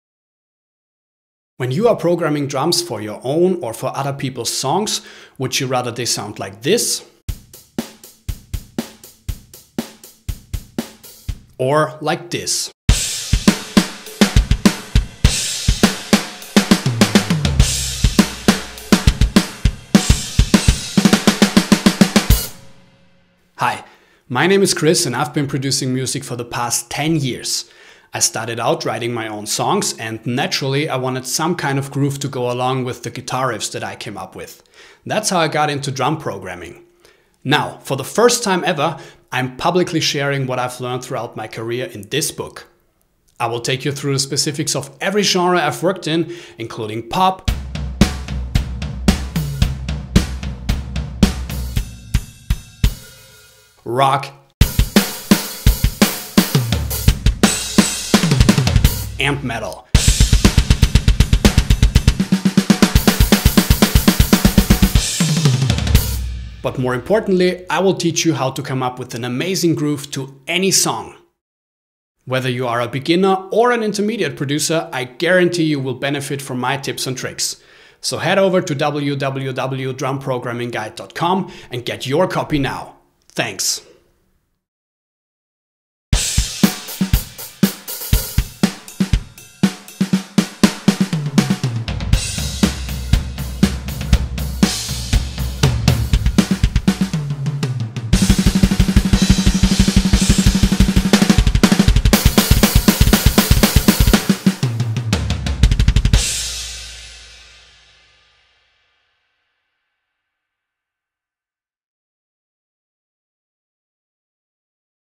HitIt - The Ultimate Guide to programming Drums - Official Book Trailer
This is the trailer for Chris Nothdurfter's book "HitIt - The Ultimate Guide to Programming Drums". Inside the book you will learn everything you need to know in order to be able to create your own natural sounding drum tracks. You will learn to think like a real drummer and how to make your tracks sound even more convincing. All techniques are covered in separate chapters for Pop, Rock, and Metal. There are also two bonus chapters about Odd time signatures and Mixing Tips.